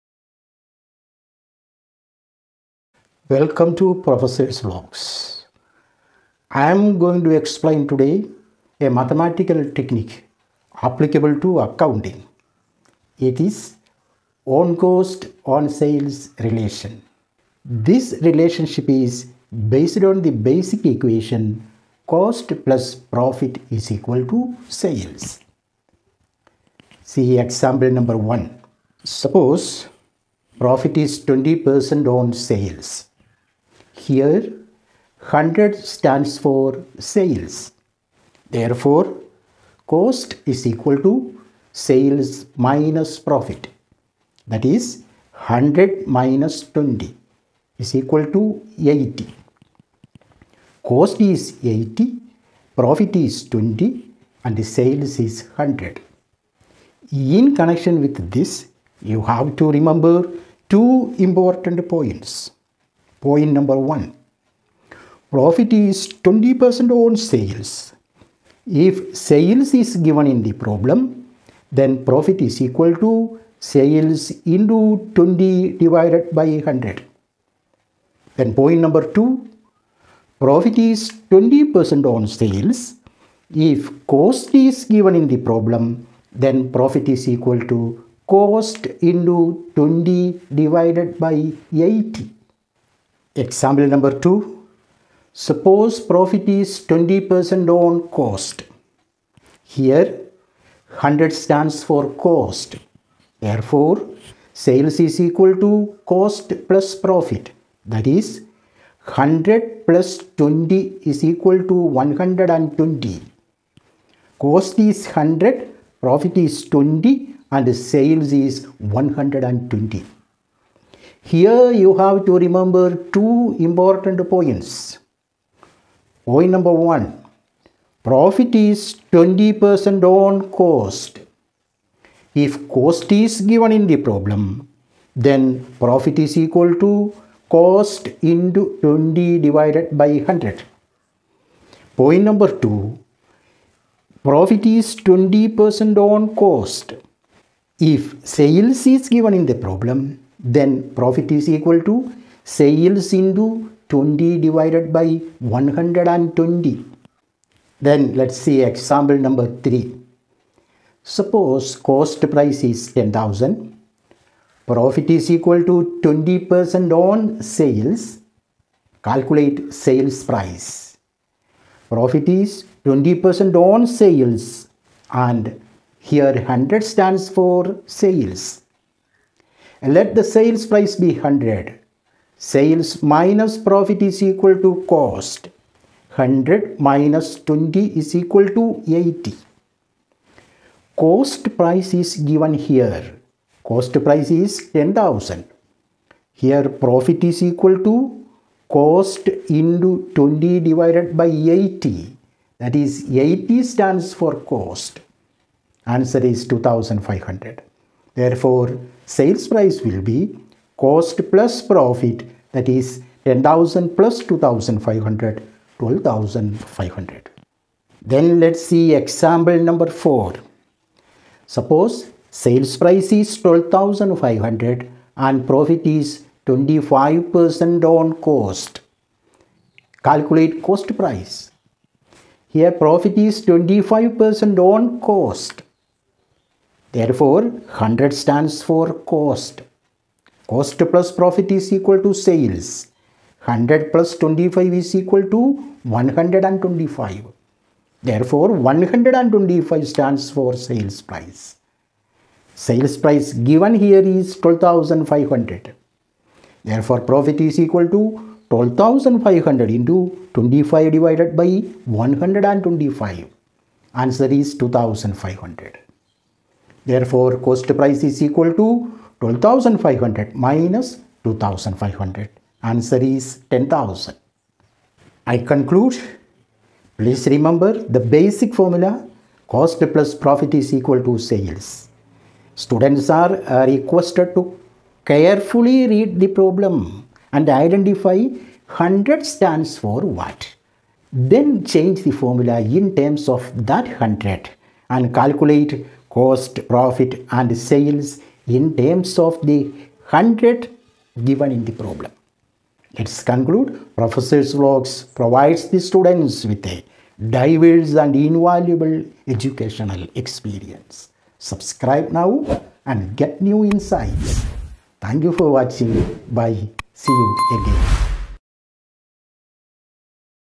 Mathematics & Accounting
Calculation of profit as a percentage on cost and on sales may normally get confused. This video clearly explains the calculation technique of profit as a percentage on cost and on sales.
Application of mathematics in accounting is really interesting. It's a matter of enjoyment and mental happiness. Do you want to relax with some mathematics? Then this video is for you.
Do you want to have a clear example of the accounting treatment of loss of stock by fire? Then this video is for you
Are you searching for the calculation procedure of manager's commission after charging such commission? Then this video is for you

Professors Vlogs is a destination for accounting tips and lessons that teaches you how to improve your accounting skills and knowledge